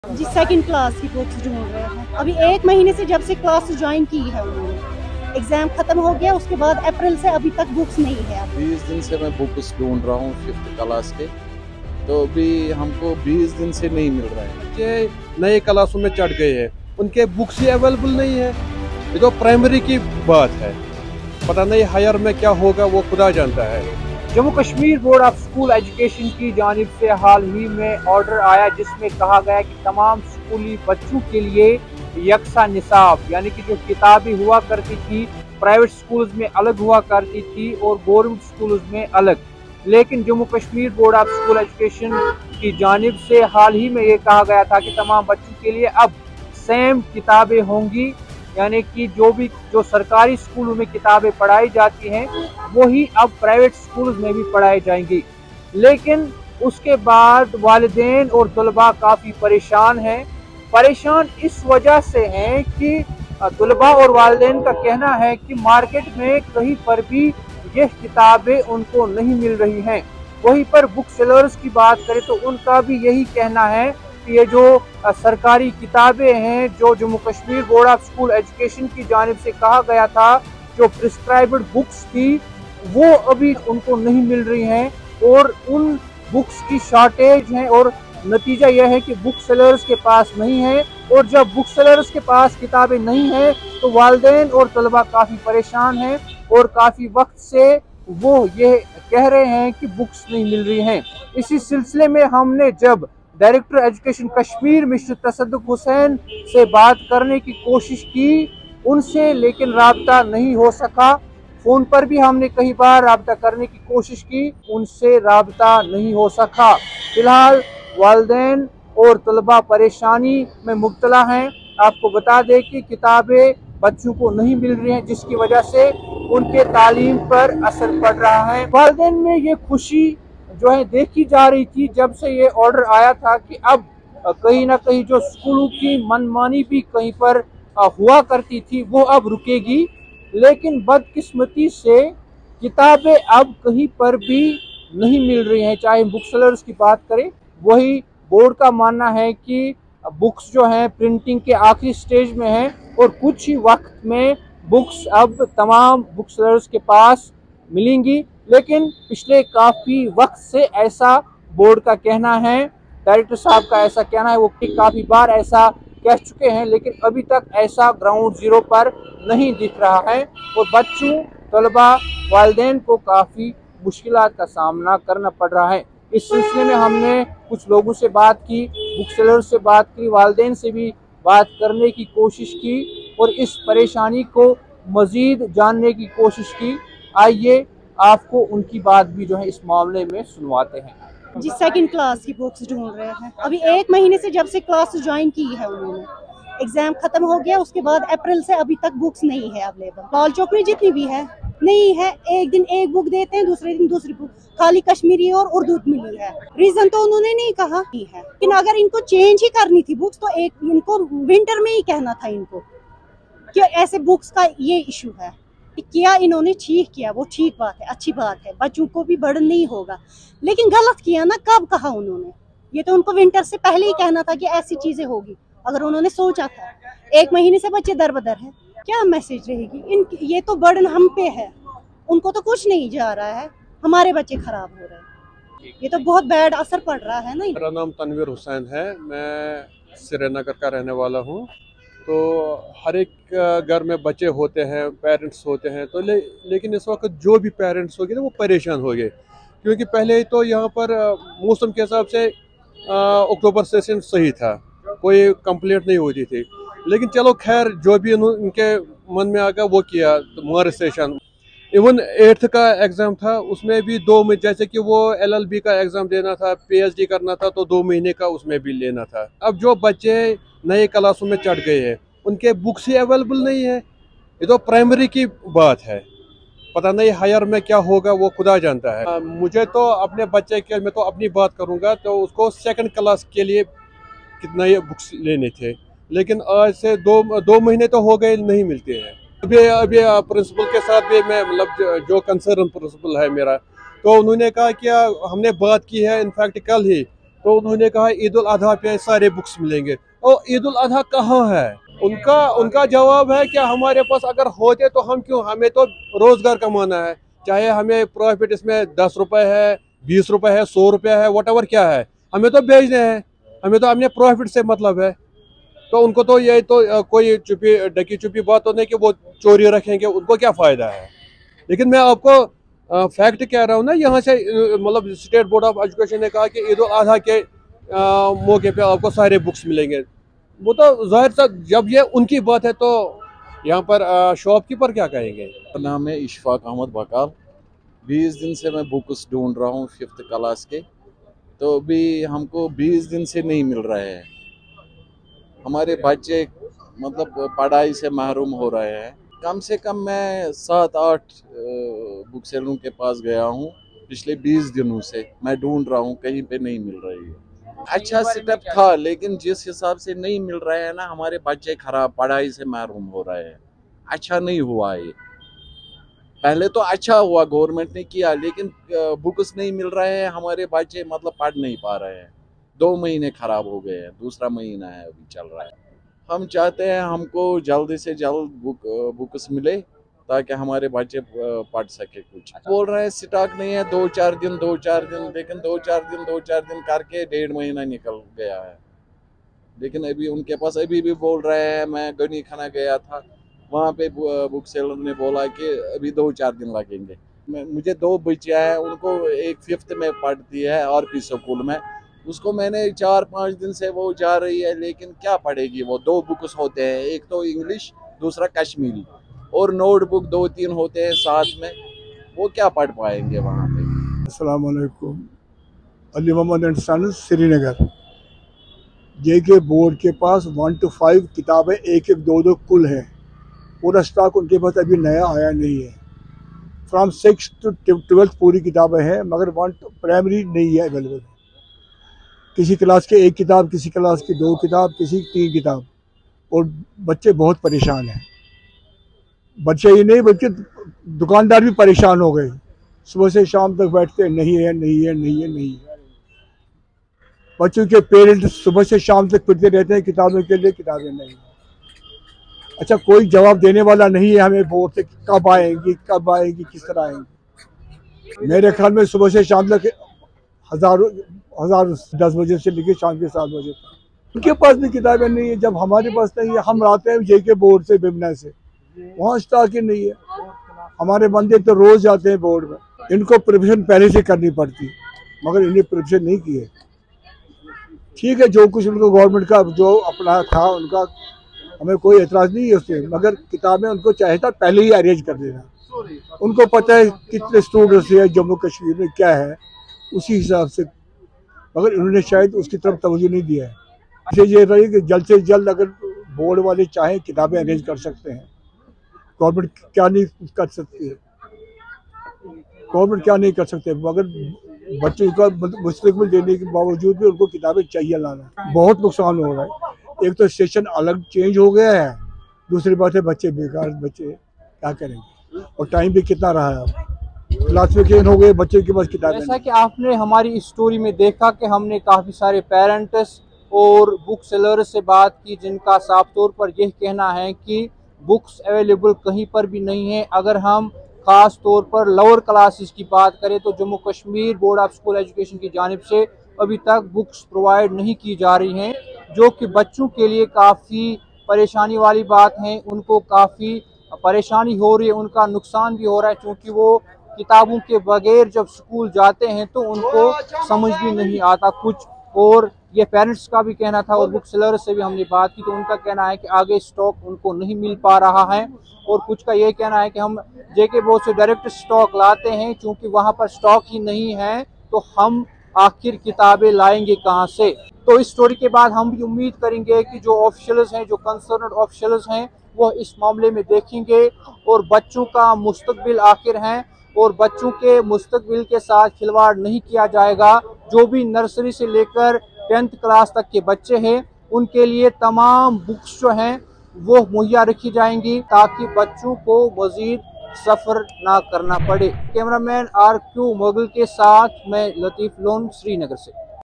Unavalibility of JKBOSE Prescribed Books on Book Stores Across Kashmir, Parents, Students aghast
watch || Unavalibility of JKBOSE Prescribed Books on Book Stores Across Kashmir, Parents, Students and Booksellers aghast, Watch a Detailed Report.

Reports: Lateef Lone
Camera: Rq Mughal